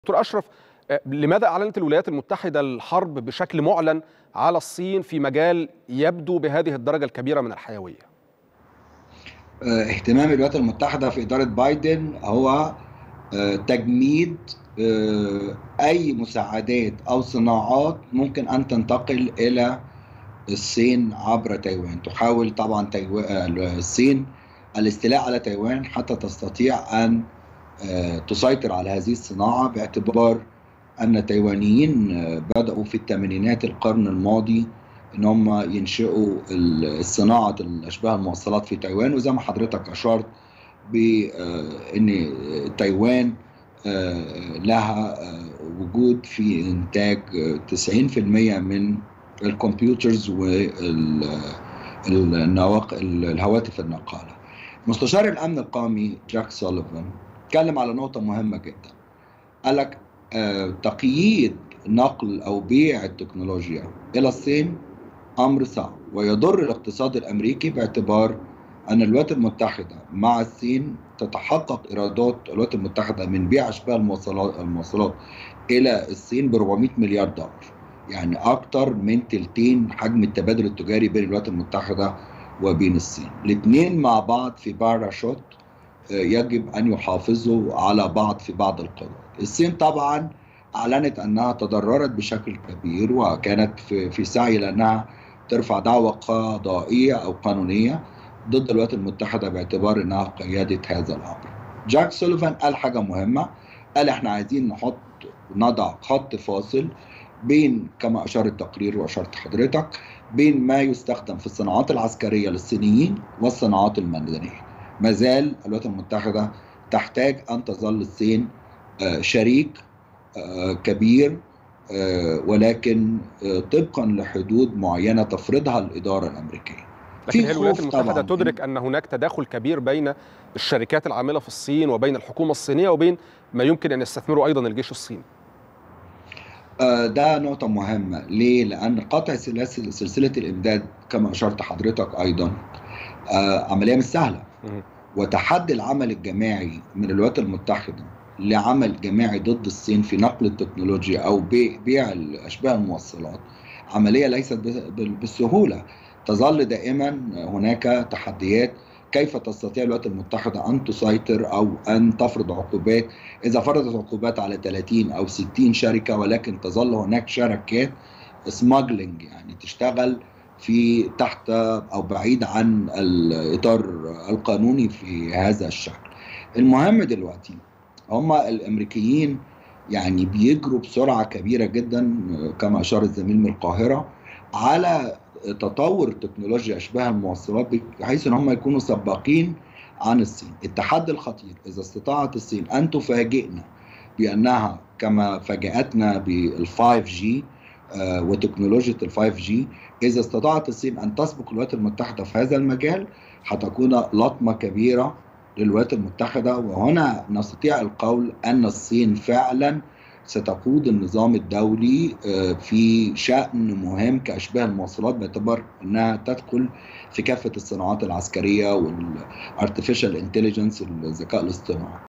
لماذا تصارع أمريكا الصين بسبب "أشباه الموصلات"؟.. د. أشرف سنجر يُجيب
15/1/2022